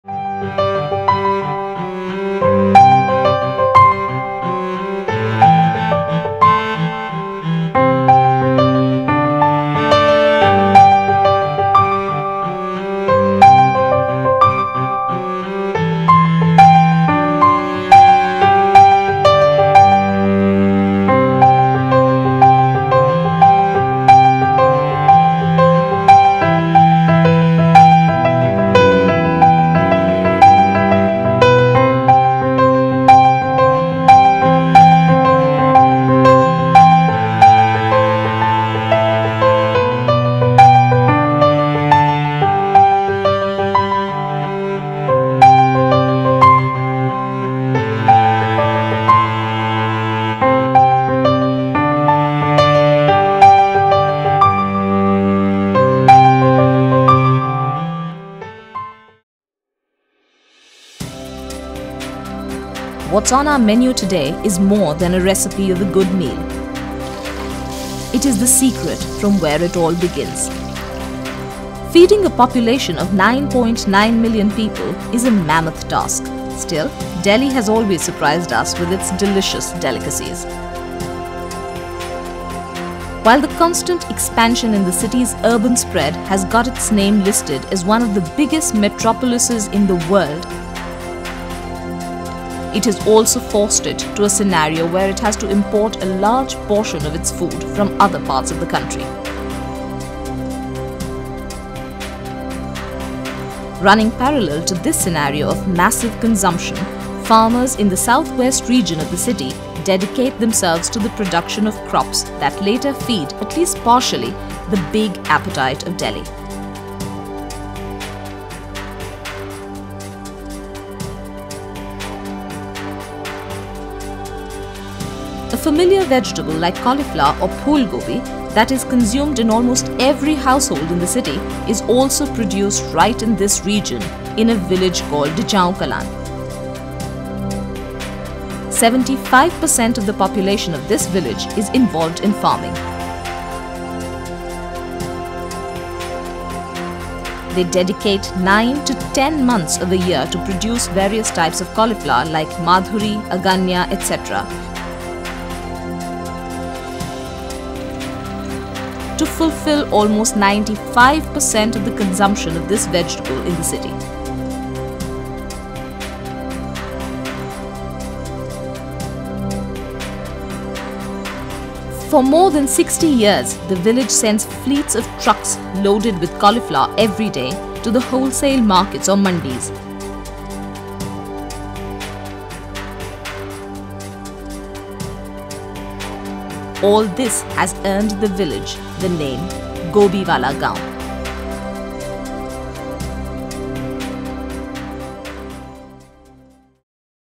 Gobhi wala Gaon - India's Cauliflower village!
What’s on our menu today is more than a recipe for a good meal.
It’s the secret from where it all begins.

Feeding a population of 26 million people is a mammoth task.
Still, Delhi has always surprised us with its delicious delicacies.

While the constant expansion in the city’s urban spread has got its name listed as one of the biggest metropolises in the world, it has also forced it to a scenario where it has to import a large portion of its food from other parts of the country.

Running parallel to this scenario of massive consumption, farmers in the south-west region of the city dedicate themselves to the production of crops that later feed, at least partially, the big appetite of Delhi.

A familiar vegetable like cauliflower or Phool gobhi that is consumed in almost every household in the city is also produced right in this region in a village called Dichaon Kalan.

75% of the population of this village is involved in farming. They dedicate 9 to 10 months of a year to produce various types of cauliflower like Madhuri, Aganya etc. to fulfil almost 95% of the consumption of this vegetable in the city.

For more than 60 years the village sends fleets of trucks loaded with cauliflower every day to the wholesale markets or mandis. All this has earned the village the name- Gobhi Wala Gaon.